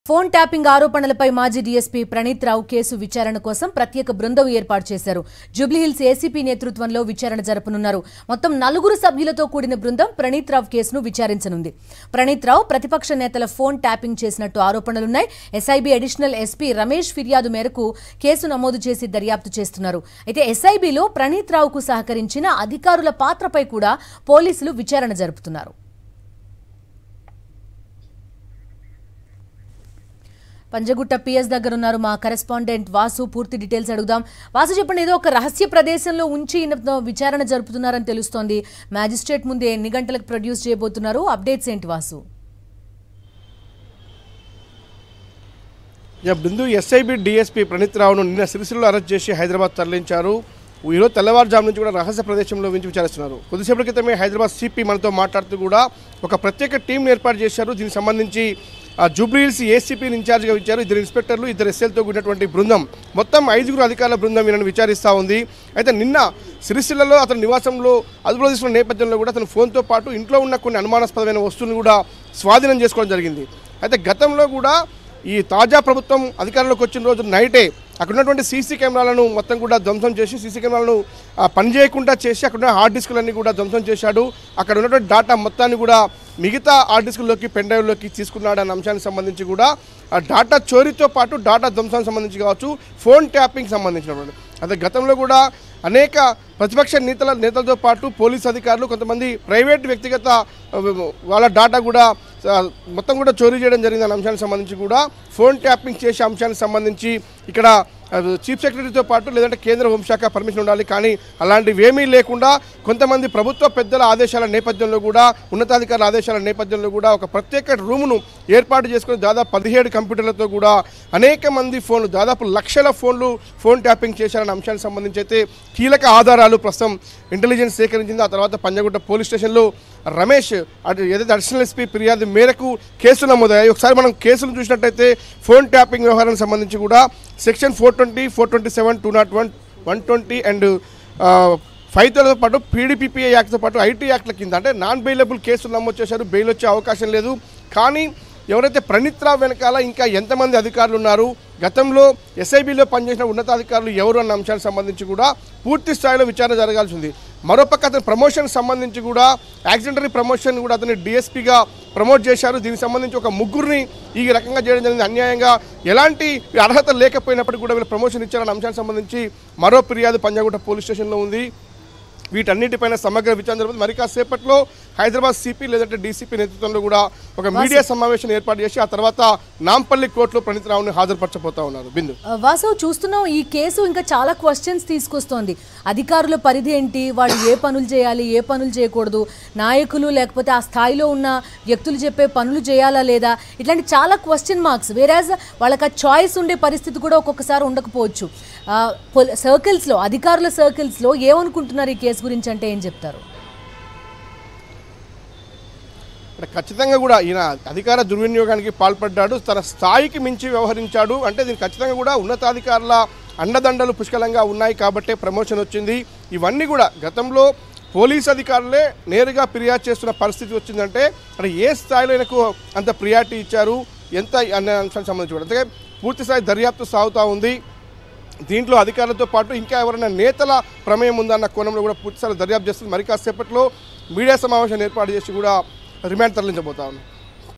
Special Team Investigating DSP Praneet Rao Case | Phone Tapping Case
In a significant development, SakshiTV brings you exclusive details on the arrest of suspended DSP Praneeth Rao in the ongoing phone tapping case. Join us as we delve into the allegations, investigate the circumstances surrounding the arrest, and analyze the potential implications for the investigation. With SakshiTV's comprehensive coverage, stay informed about the latest developments in this unfolding scandal. Tune in now for the full story!